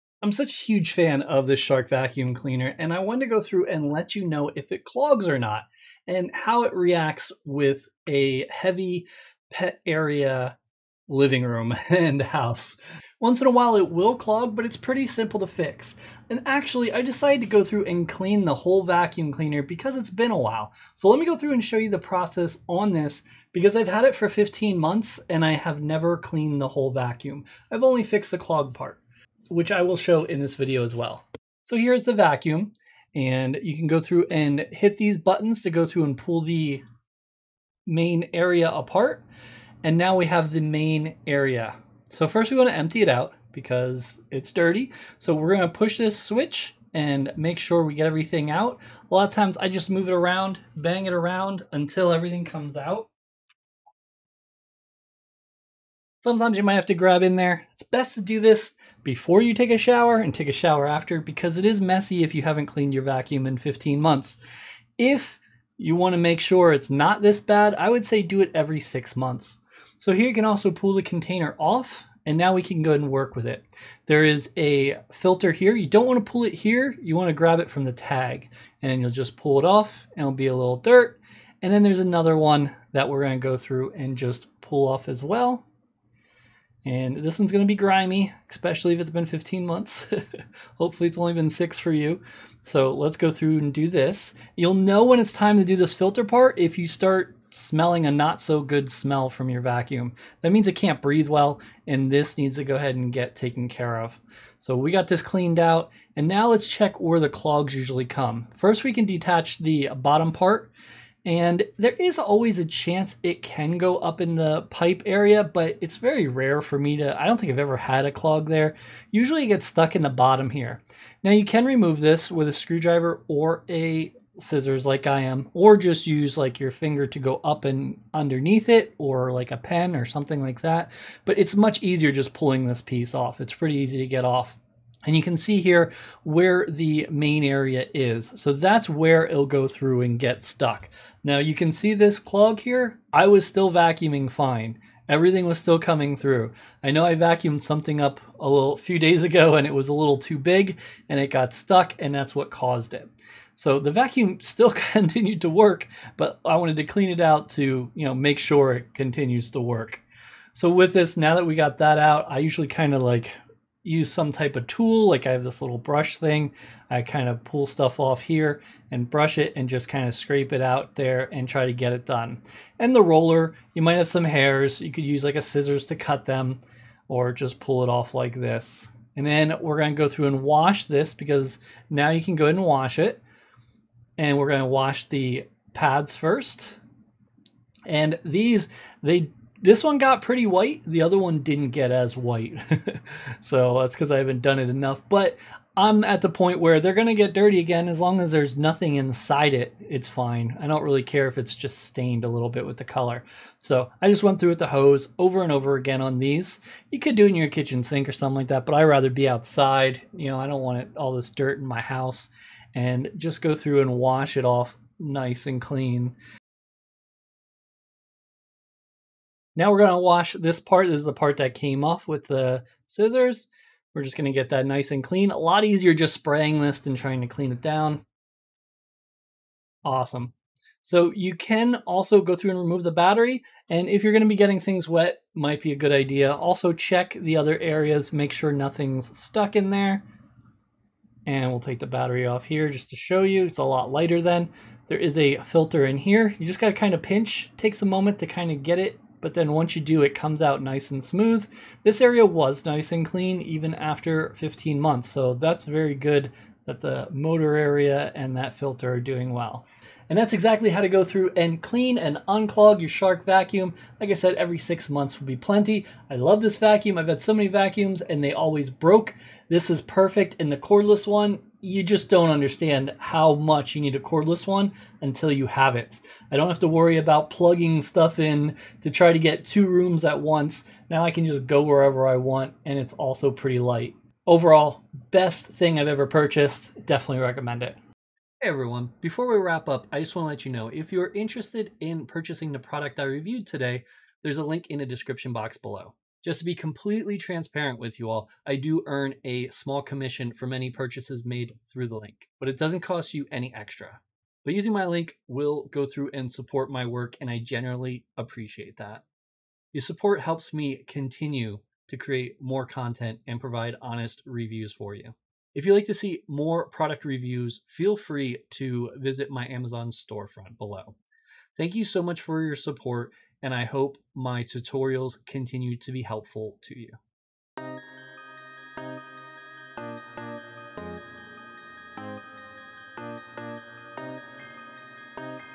How To Clean The Shark IZ361H Pet Plus Anti-Allergen Cordless Stick Vacuum
Featured Product: Shark IZ361H Pet Plus Anti-Allergen Cordless Stick Vacuum
Check Price On Amazon